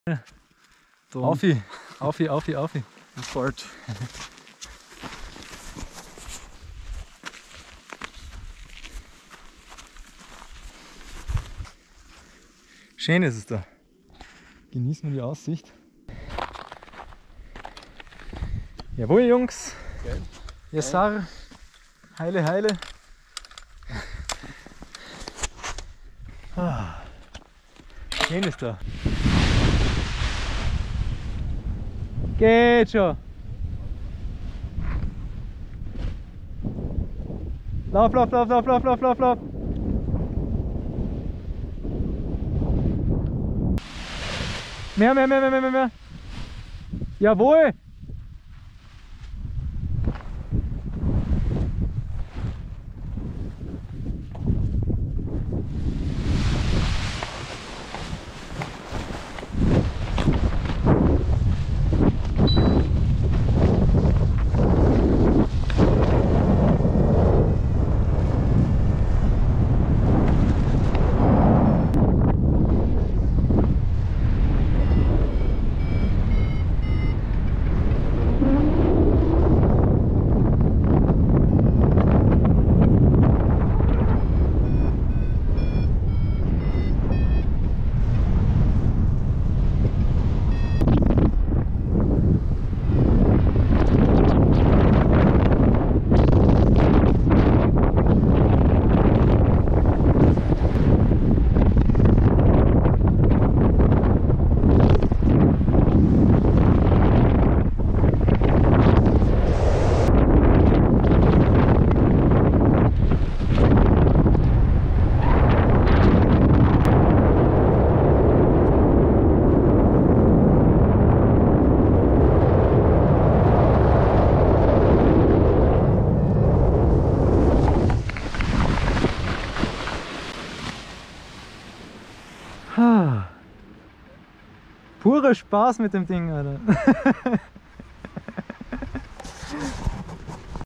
Hike & Fly Lärchenkogel (Niedere Tauern)
A nice late autumn Hike & Fly with a Skywalk X-Alps 4, thanks Geri!